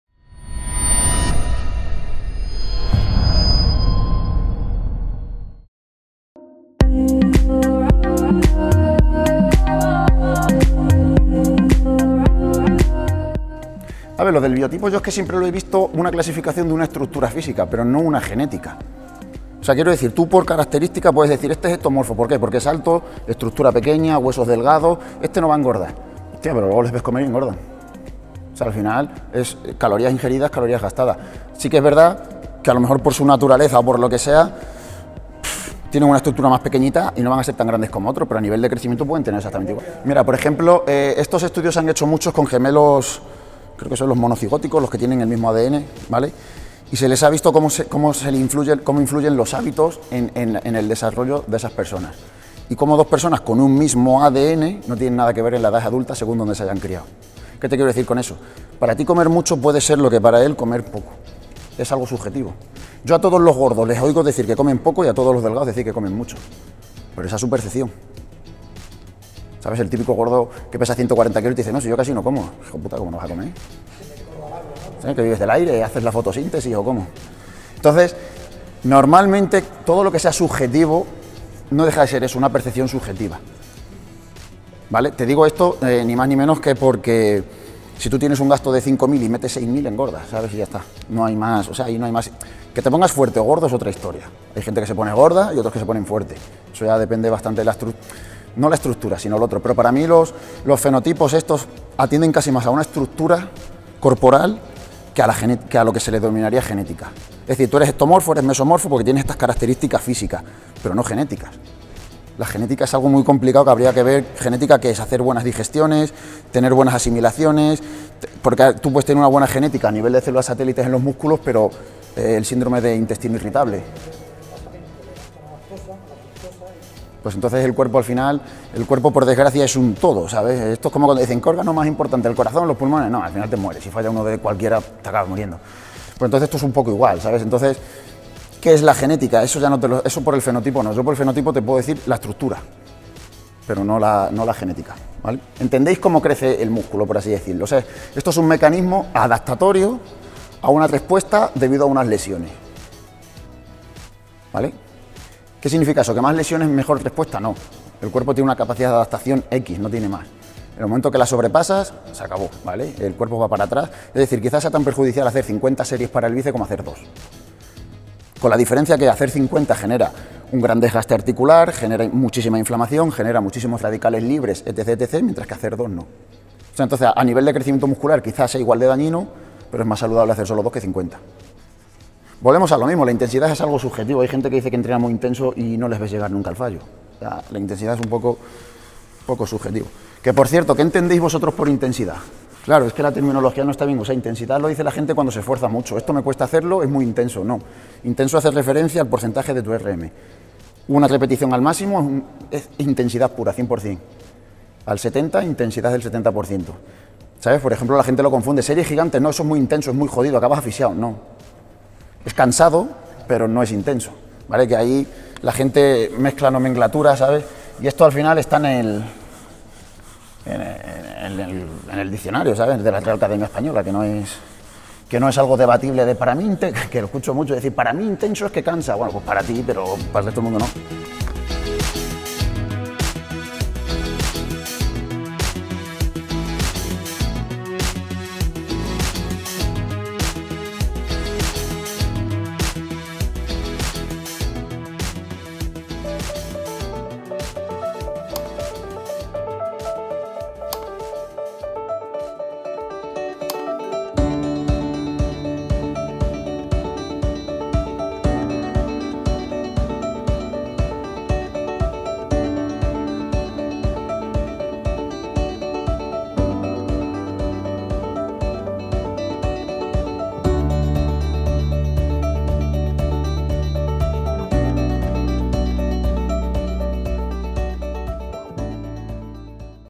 🆗 La VERDAD sobre los BIOTIPOS y la GENÉTICA 👌 [ + aclaraciones ]
Robert Castellano nos habla sobre los biotipos.